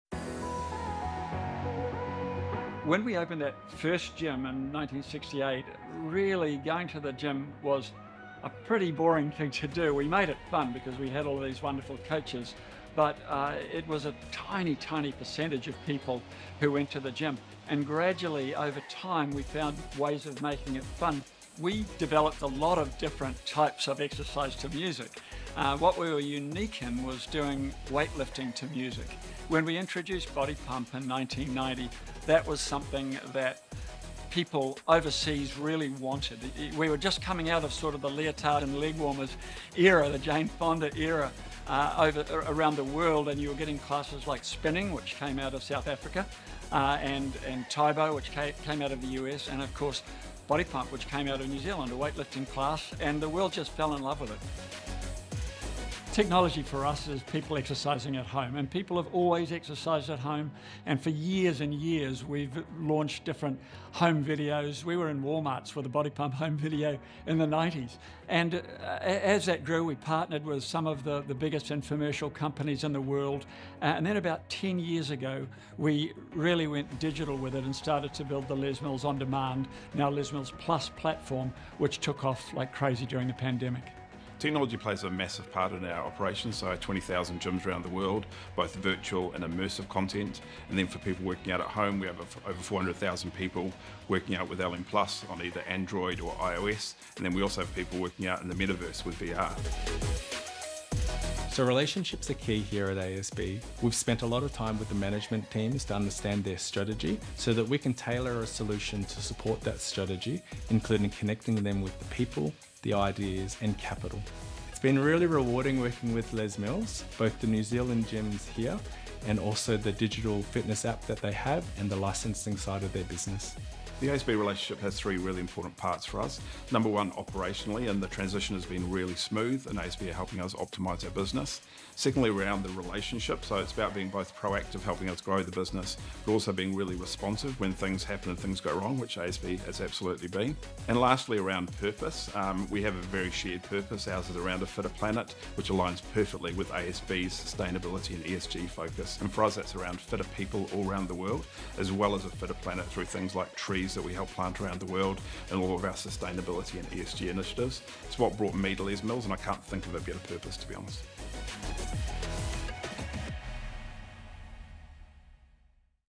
For a fitter planet | ASB
Watch how Les Mills started as a Grey Lynn gym and evolved into a global fitness powerhouse. Learn how innovation, technology and purpose are transforming customer experiences and helping to create a fitter, greener planet. For more business tips, tools, templates and support, visit the ASB Business Hub